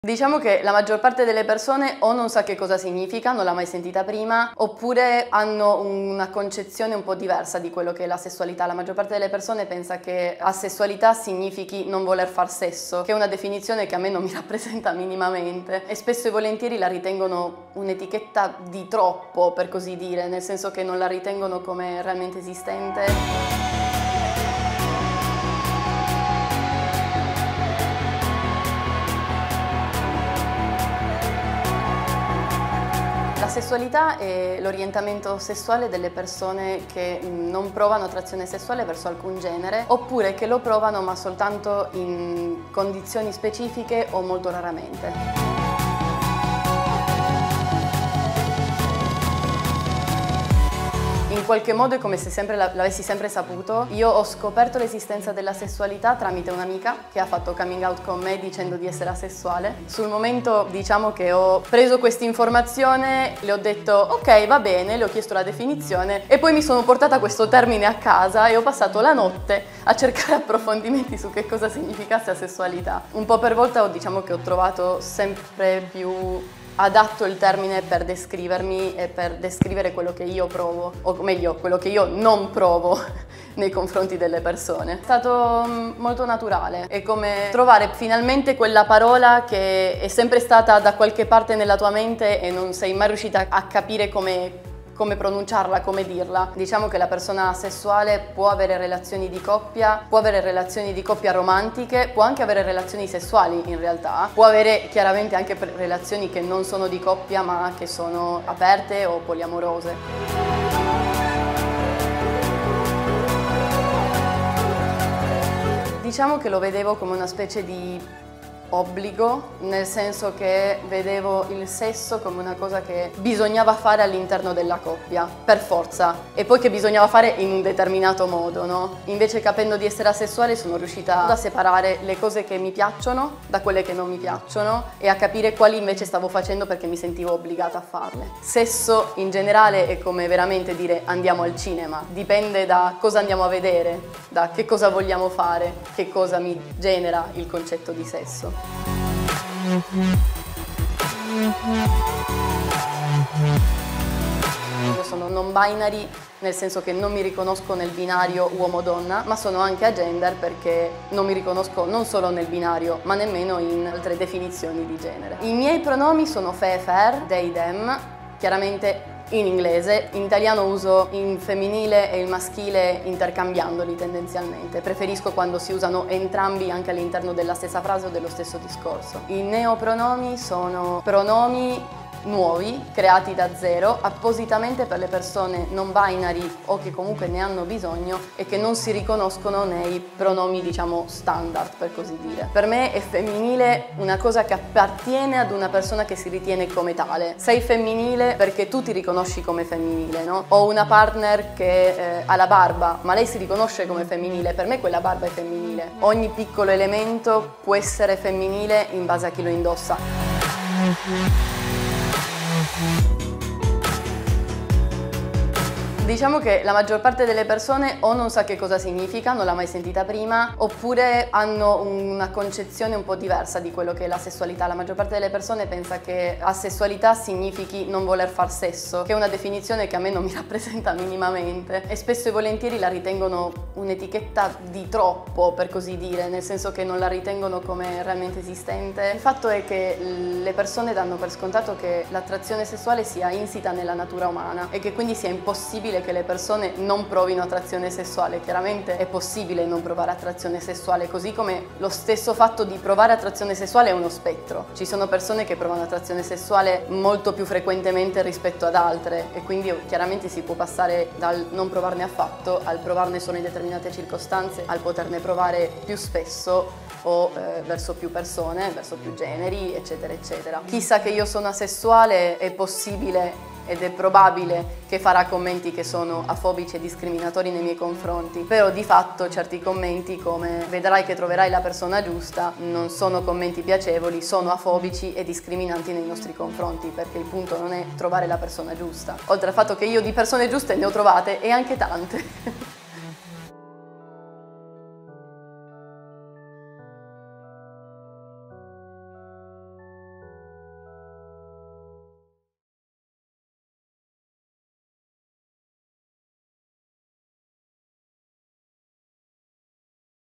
Constanza spiega cos'è l'asessualità: "È possibile non provare attrazione sessuale"
Constanza Pelissero ha spiegato a Fanpage.it il significato di asessualità: "È l'orientamento sessuale di coloro che non provano attrazione sessuale verso alcun genere o la provano solo in condizioni specifiche, molto raramente". Lei ha fatto coming out l'1 giugno 2018, riuscendo finalmente a dare un nome a qualcosa che in fondo di sé aveva sempre saputo, ma che non riusciva a definire. Da quel momento in poi si è sentita continuamente rivolgere frasi come: è solo una fase, sei ancora troppo giovane, devi solo trovare la persona giusta. "Non sono commenti piacevoli, sono commenti afobici e discriminanti" ha spiegato. Il suo orientamento sessuale è poco conosciuto, è quasi un tabù: "La maggior parte delle persone o non sa cosa significa, non l'ha mai sentita prima, oppure pensa che asessualità significhi non voler far sesso. Spesso la ritengono un'etichetta di troppo per così dire, non veramente esistente".